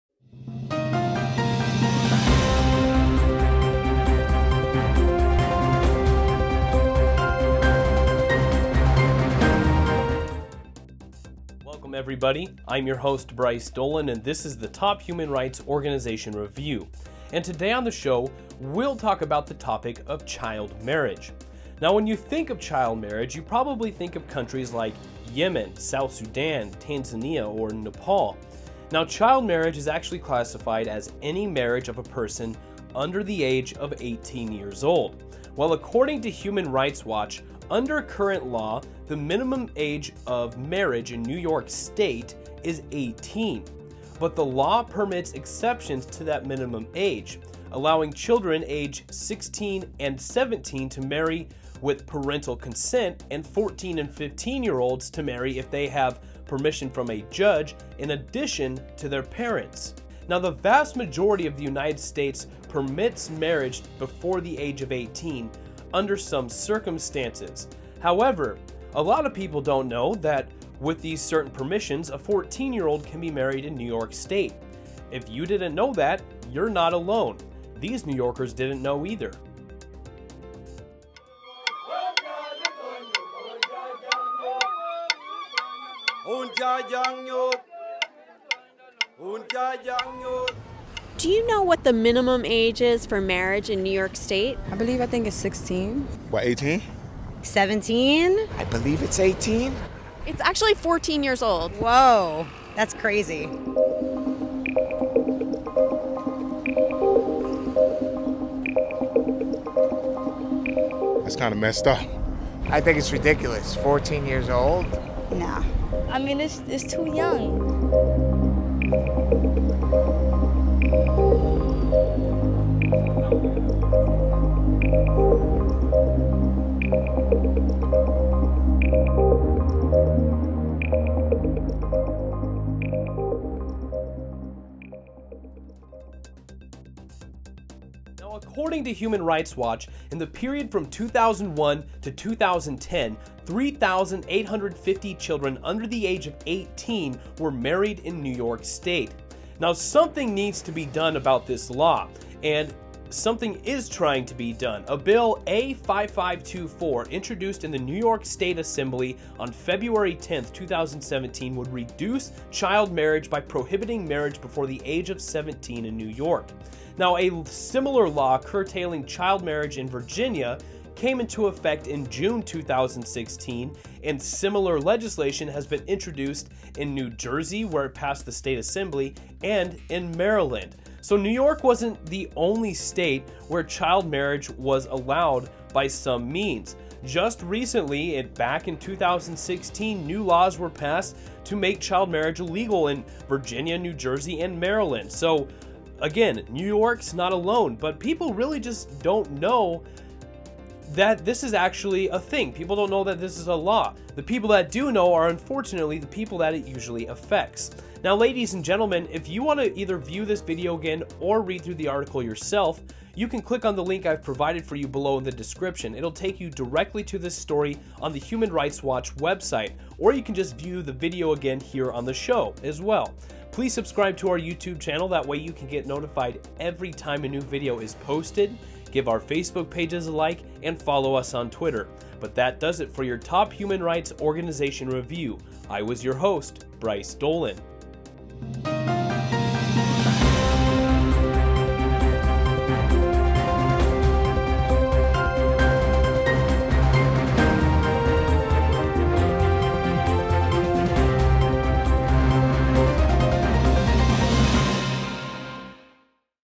EndChildMarriage in New York, yes this is a real issue
Child marriage is legal in a number of different countries, including the United States. Child marriage is any marriage of a person under the age of 18. Over 3,000 children under the age 18 married in New York from 2001 to 2010.

Advocate for bill A.5524 to help EndChildMarriage in New York.